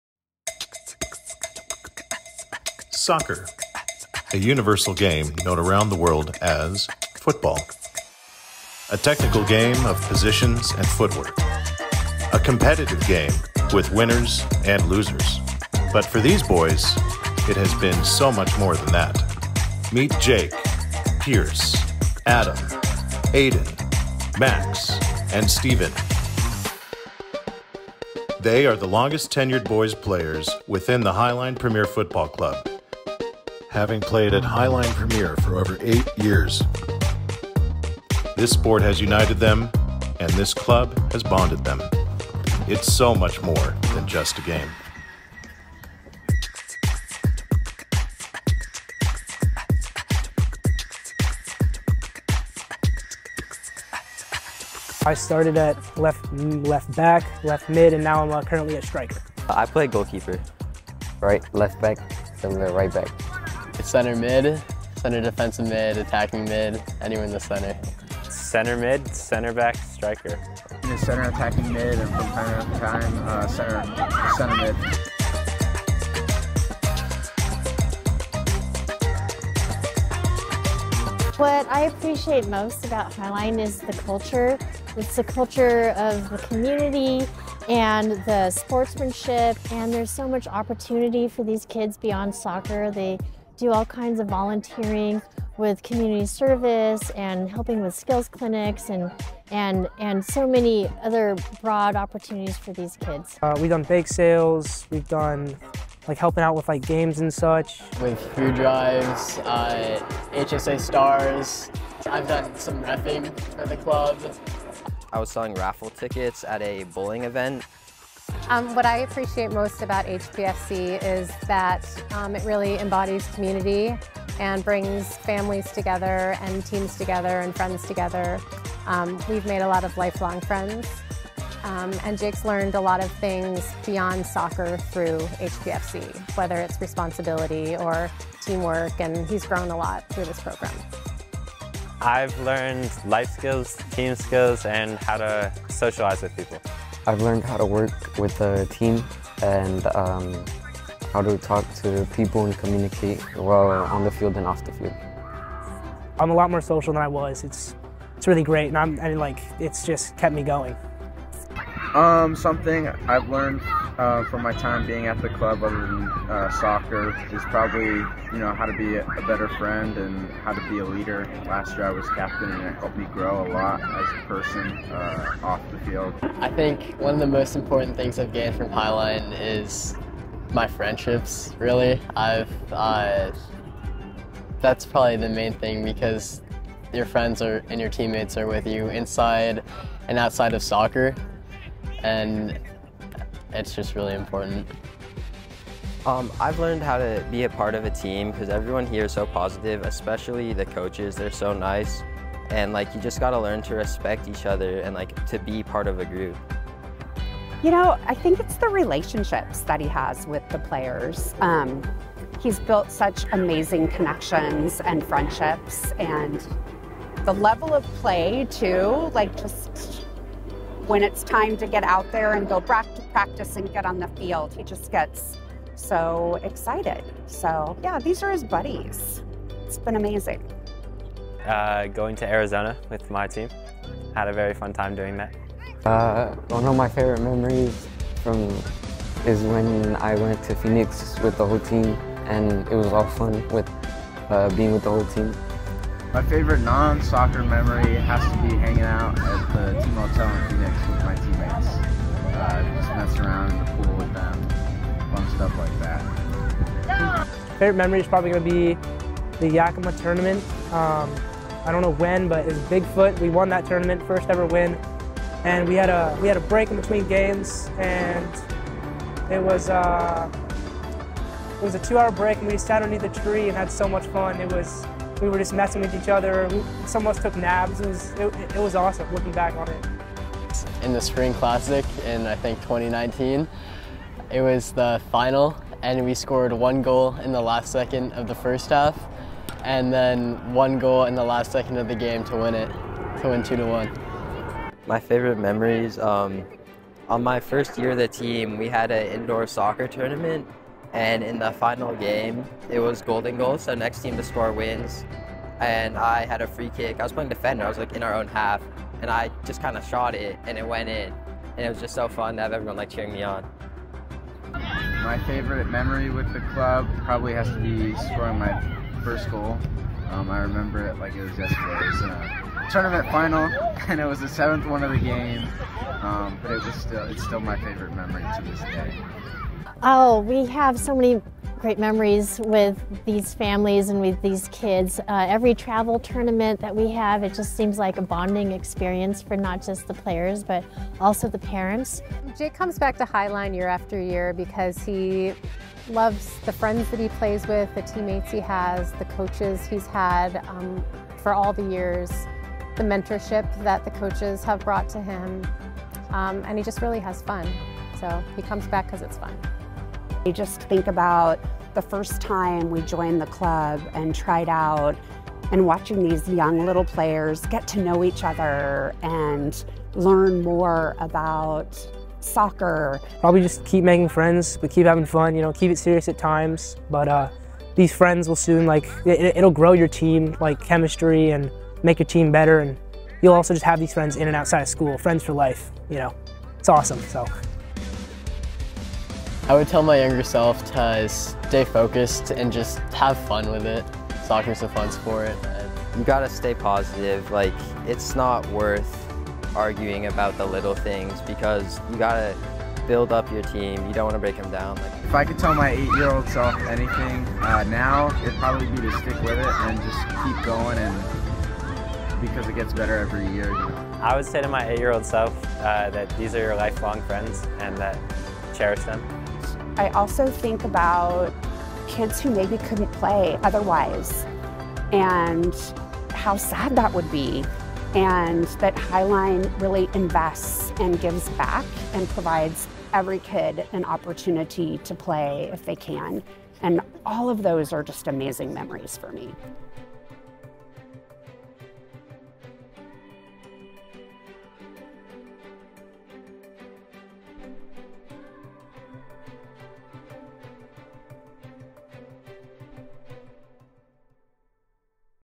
HPFC Auction Video 2023 FINAL
For the 2023 Fund a need portion of the HPFC auction, Highline Premiere Football Club featured 6 of the 07 boys, who have played with this club since they were 8 years old telling their inspiring story and sharing their favorite memories.